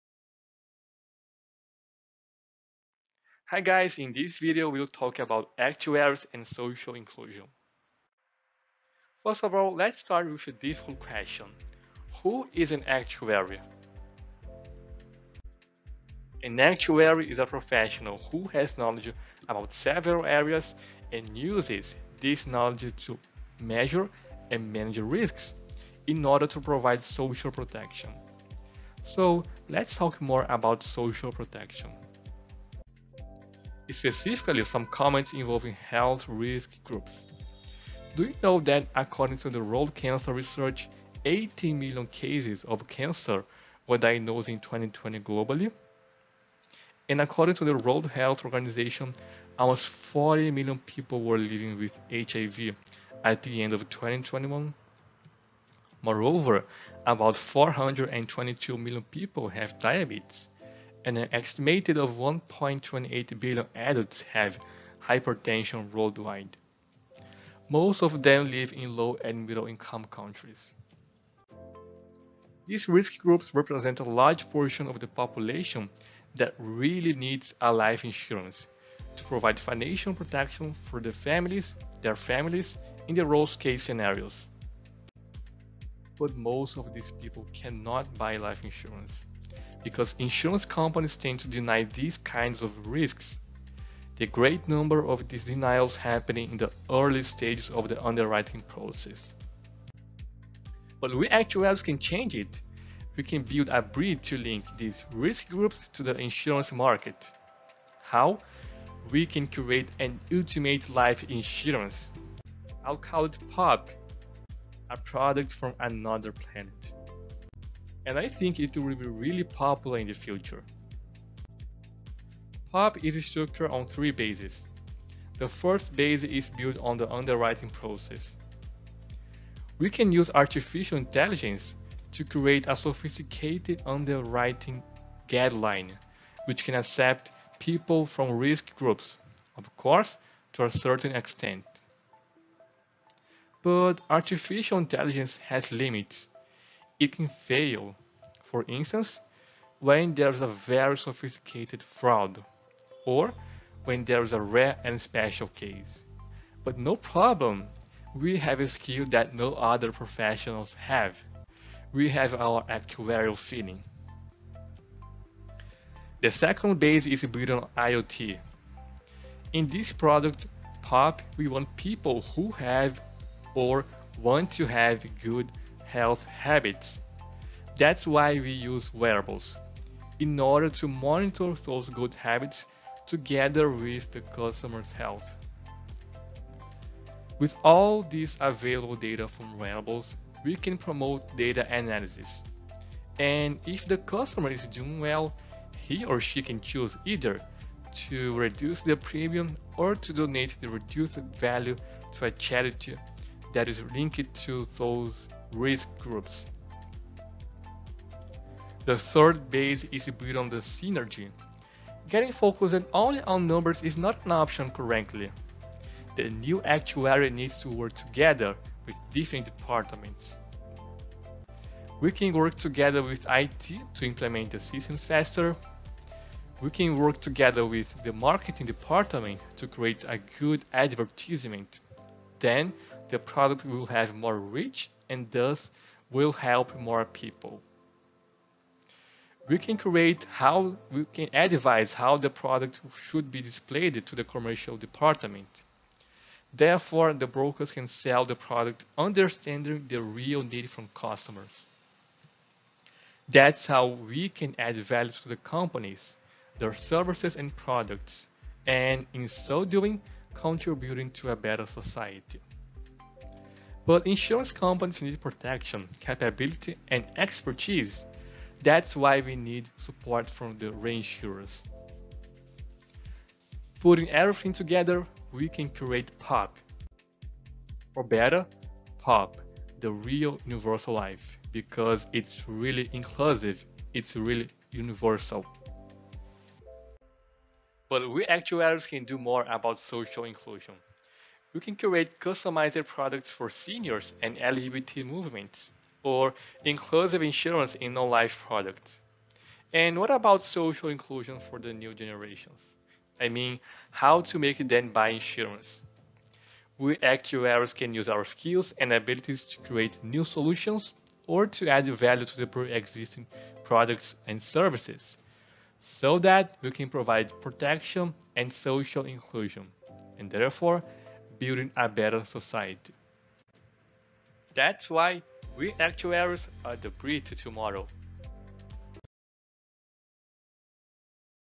YAWC video 15. The Bridge to Tomorrow: Actuaries and Social Inclusion
Actuaries are skilled professionals who have knowledge about several areas. Their main responsibility and purpose is to use all these knowledge and skills to provide social protection.
People from the following risk groups: diabetes, hypertension, HIV and cancer, represent a large portion of the population that really needs life insurance to provide financial protection for their families. But, most of these people cannot buy life insurance, because insurance companies tend to deny these kinds of risks.
However, actuaries can change it. They can build a bridge to link these risk groups to the insurance market.
They can use their skills and abilities to create new solutions, or to add value to pre - existing products and services so that they can provide protection and social inclusion, and, therefore, build a better society.